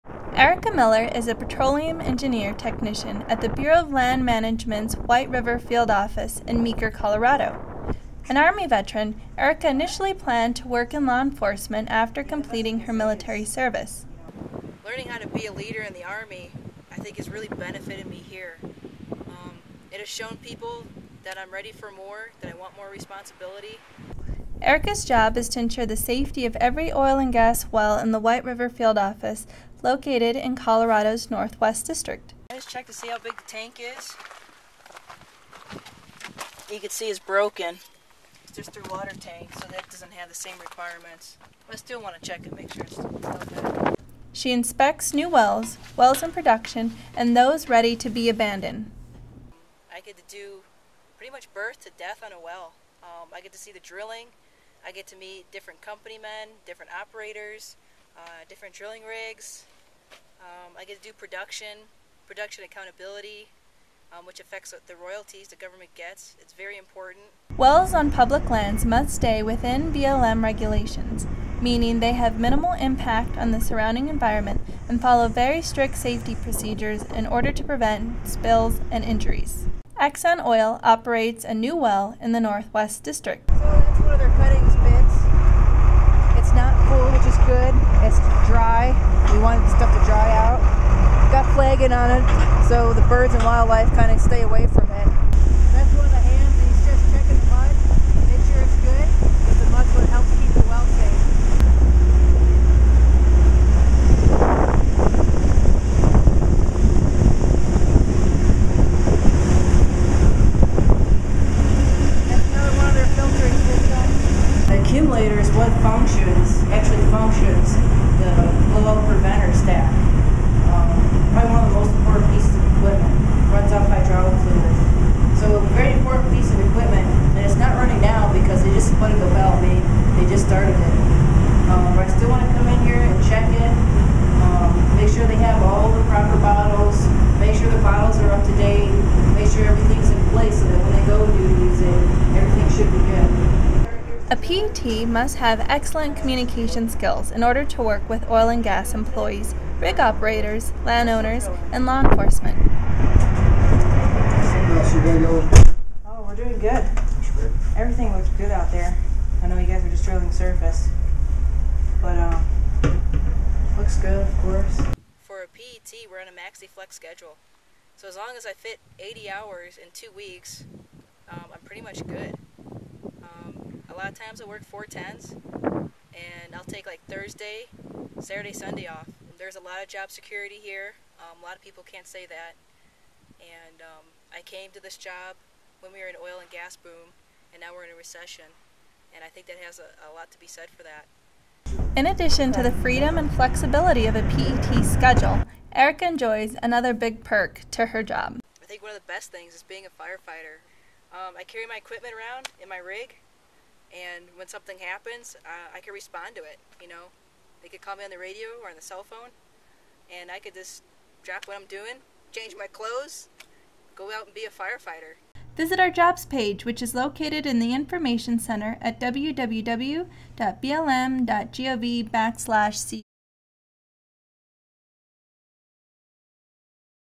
BLM Recruitment Project: Erika Miller
Erika Miller, Petroleum Engineering Technician in the White River Field Office, Meeker, Colo., talks about her job with the Bureau of Land Management.